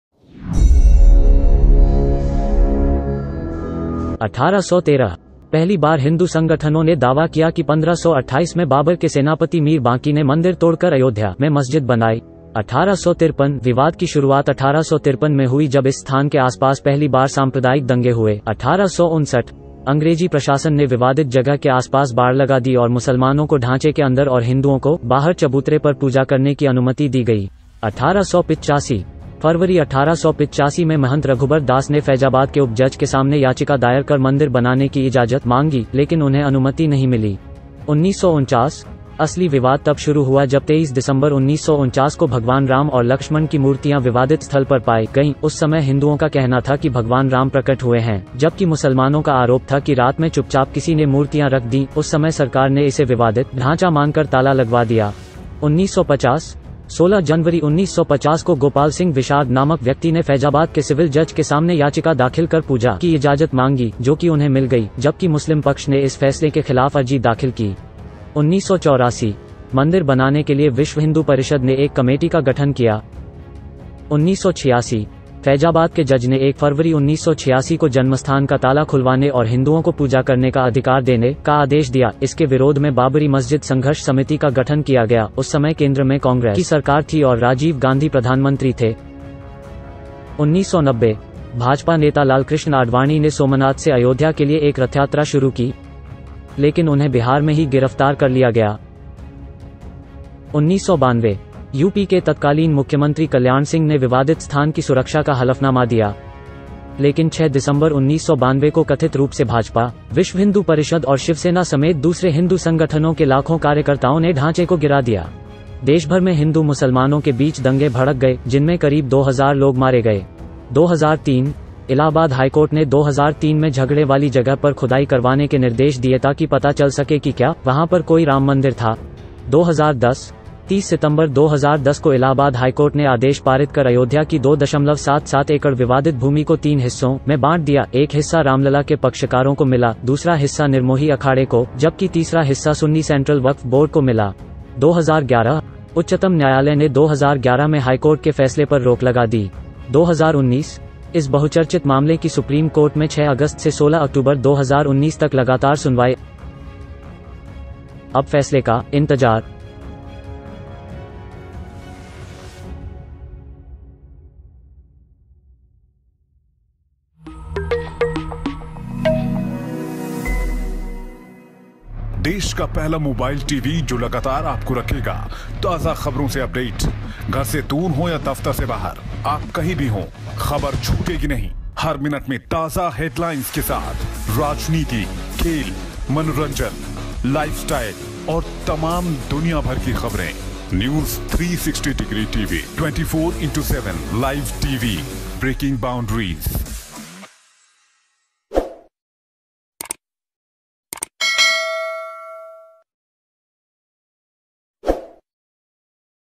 Ayodhya Verdict : जानिए 106 साल पुराने विवाद का संपूर्ण घटनाक्रम सिर्फ 2.38 मिनट में - News 360 Tv
Ayodhya Verdict (अयोध्या) : जानिए 106 साल पुराने विवाद का संपूर्ण घटनाक्रम सिर्फ 2.38 मिनट में - News 360 Tv
Ayodhya: know the whole incident of 106 year old dispute in just 2.38 minutes - News 360 Tv

❤ Don't be a stranger, drop us a line in the comments and let us know which videos you'd like us to make next.